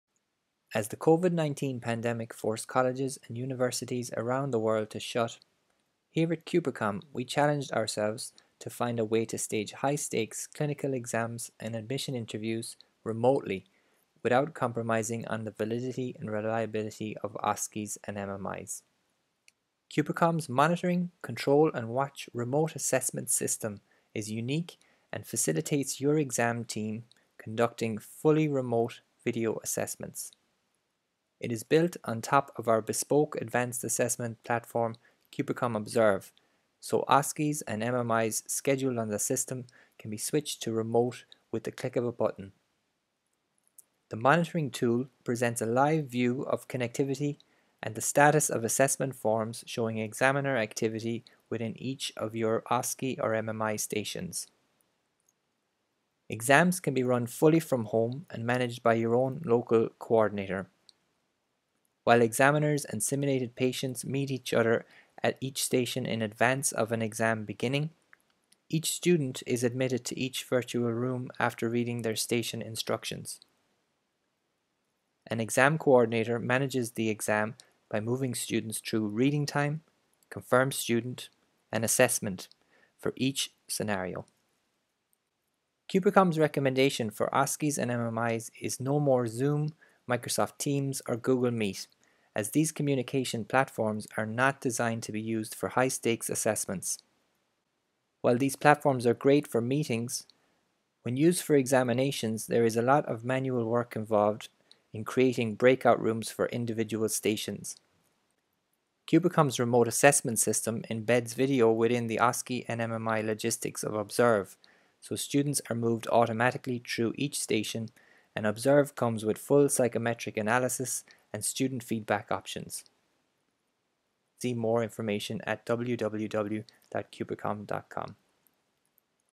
Running a Virtual OSCE [with Qpercom Observe]
Qpercom's assessment platform Observe now adds video integration to facilitate online and virtual OSCEs during the COVID-19 pandemic.

See how our system manages the OSCE examination process.